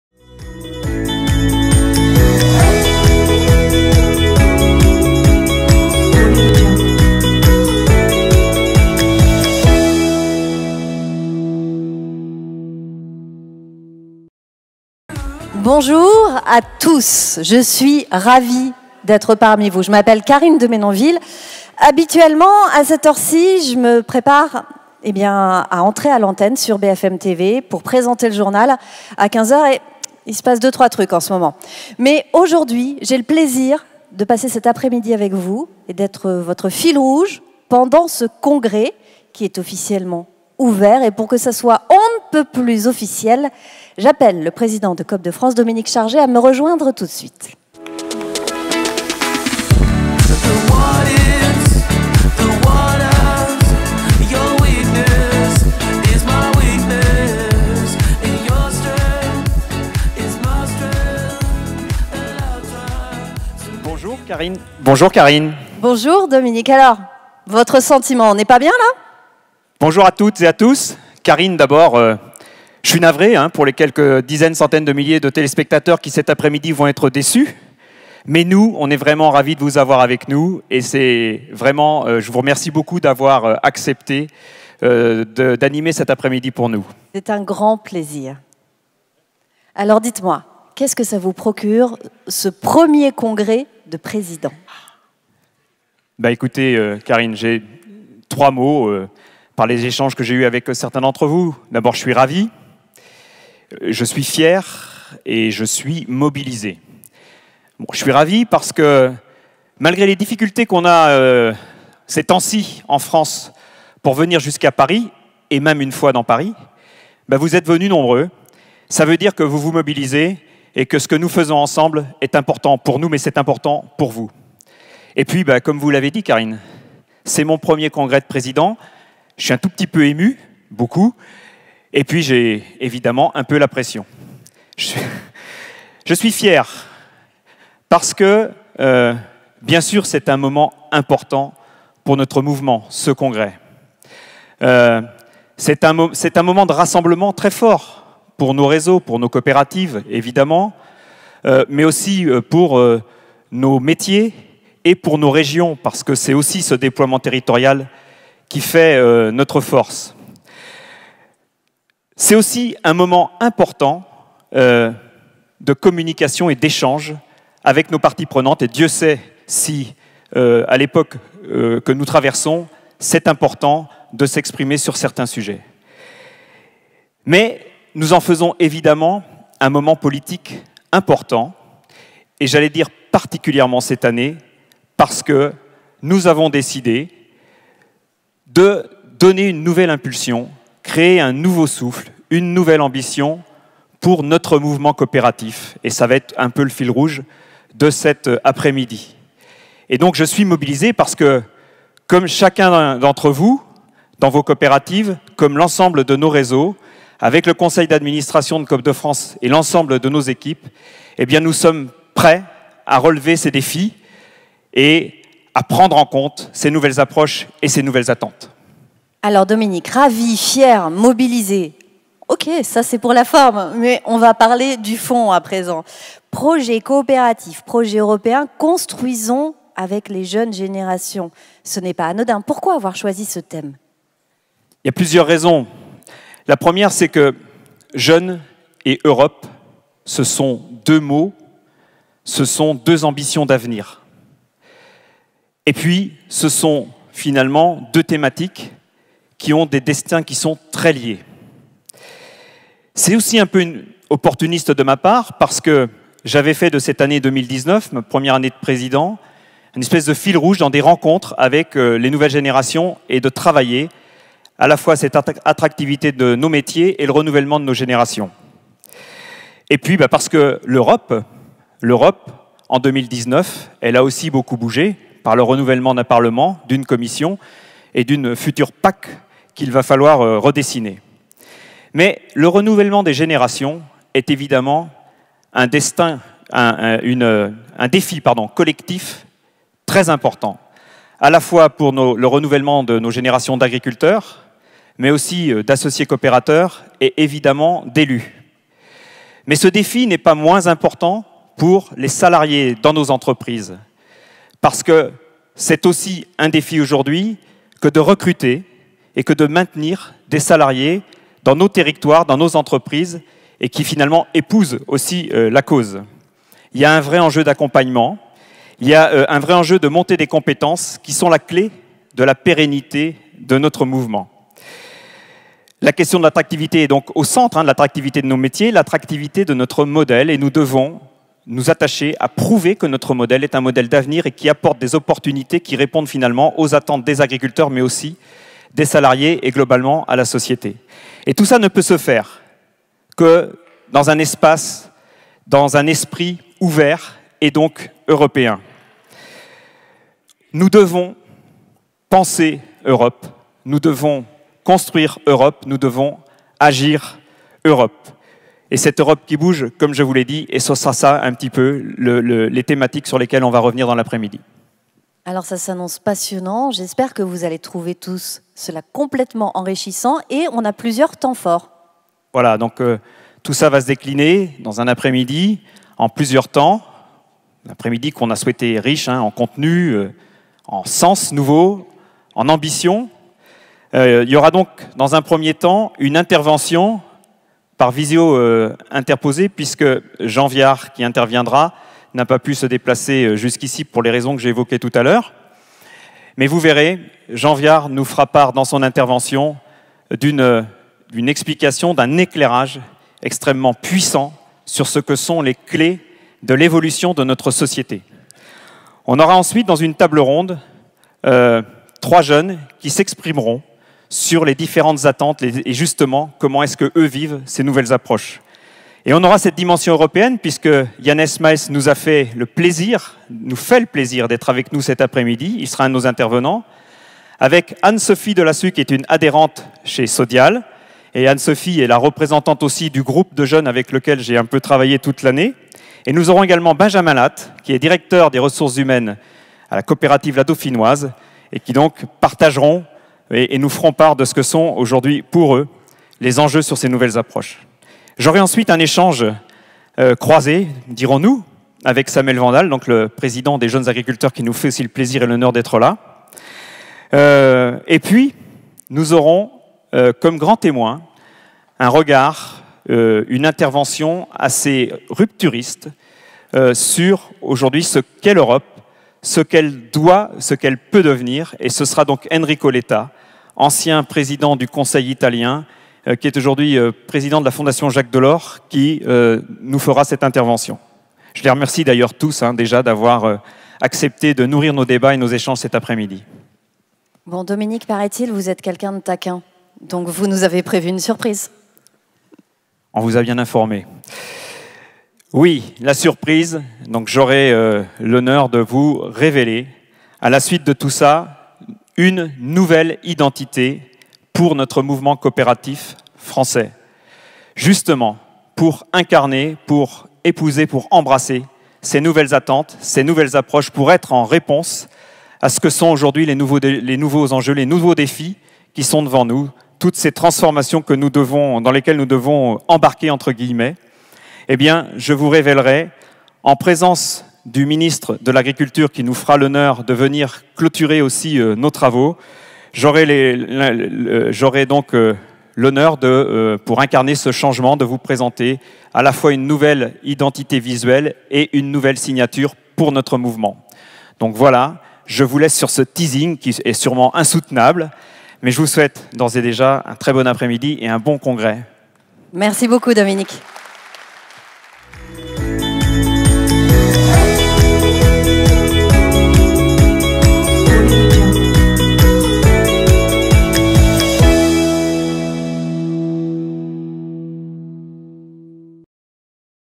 Coop de France - Congrès 2019 : ouverture des travaux
Congrès de Coop de France : "Projet coopératif, projet européen : construisons avec les jeunes générations", animé par Karine de Menonville
Ouverture des travaux par Dominique Chargé, Président de Coop de France